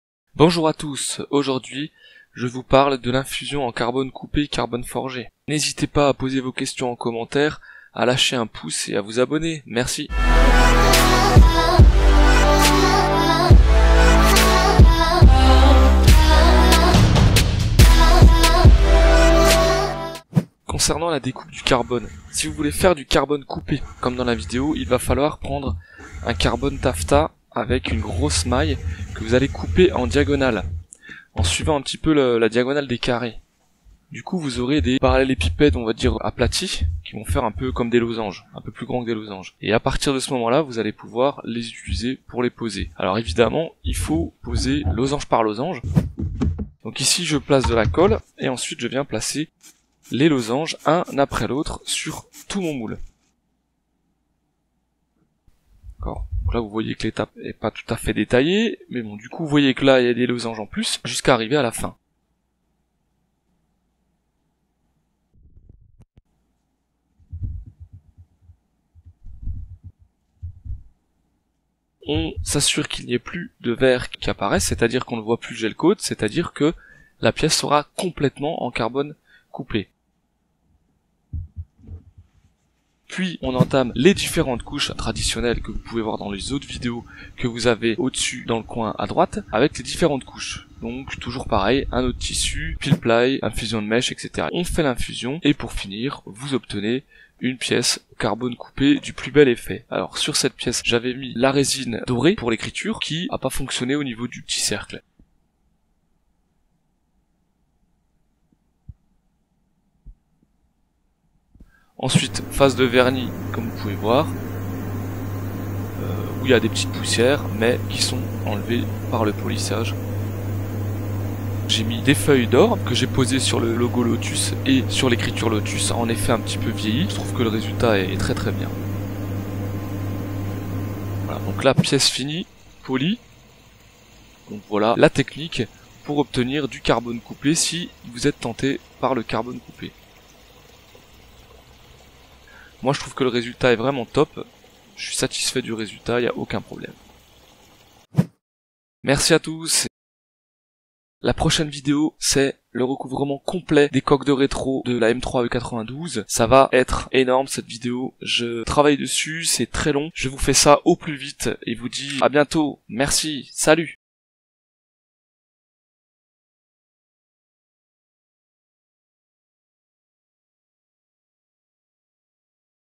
[DIY] How to make cut/forged/chopped carbon fiber? (Lotus part)
Dans cette vidéo, je vous montre comment faire une infusion en fibre de carbone coupé/forgé (carbon fiber chopped/forged) d'un cache culbuteur de Lotus Elise/Exige S2, avec lettrage en feuille d'or, sous forme de tutoriel, avec trucs et astuces. Le procédé d'infusion est le même quelque soit les pièces à faire. Une vidéo sur la fabrication d'un outillage arrivera.

Sommaire
00:00 Intro
00:20 Découpe
00:46 Collage
01:23 Infusion
01:33 Démoulage
01:53 Vernis
02:12 Polissage
02:29 Pièce finie

La vidéo est courte et montre principalement comment préparer et placer la fibre de carbone coupé dans le moule.

De nouvelles vidéos sont déjà en réflexion, avec des tests techniques, des tests matériels, l'explication des différents matériaux, des tutoriels, du DIY, de la théorie, des bons plans. du composite fibre de carbone!